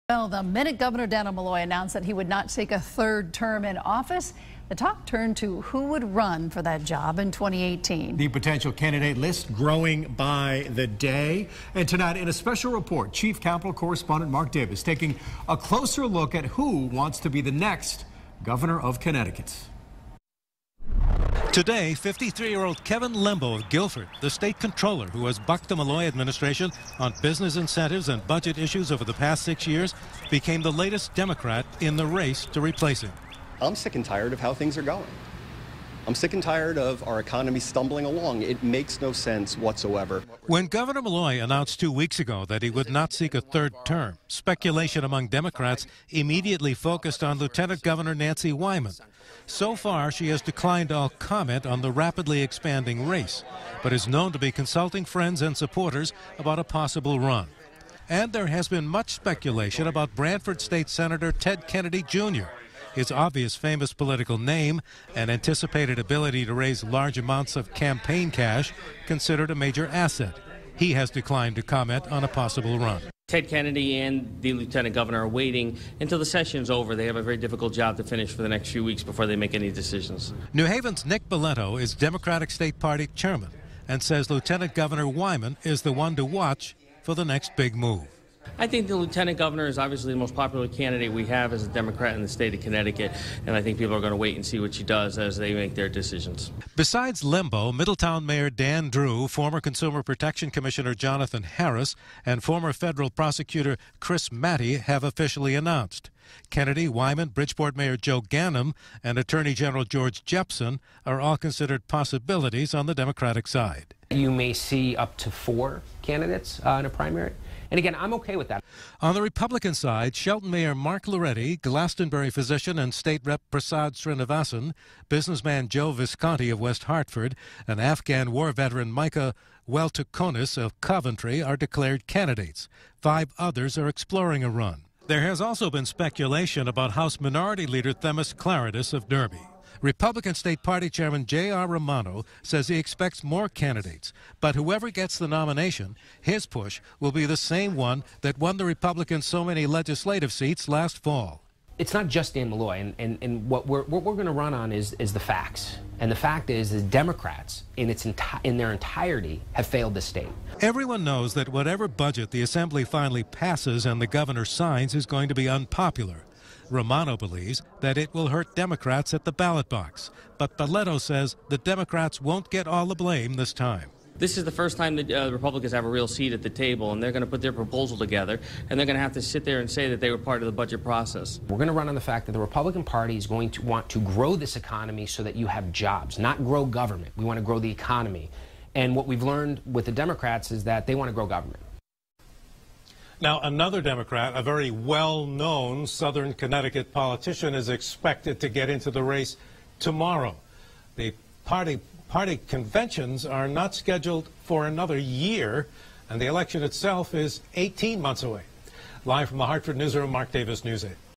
Democratic Chairman: Wyman the one to watch
The potential list of candidates in the race to replace Democratic Governor Dannel Malloy is starting to grow by the day.